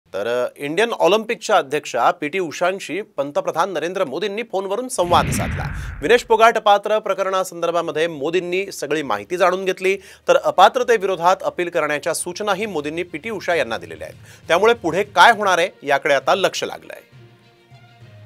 Narendra Modi यांची IOAच्या अध्यक्ष P. T. Usha यांच्याशी Vinesh Phogat अपात्र प्रकरणासंदर्भात चर्चा
Narendra Modi's discussion with IOA President P. T. Usha regarding Vinesh Phogat disqualification case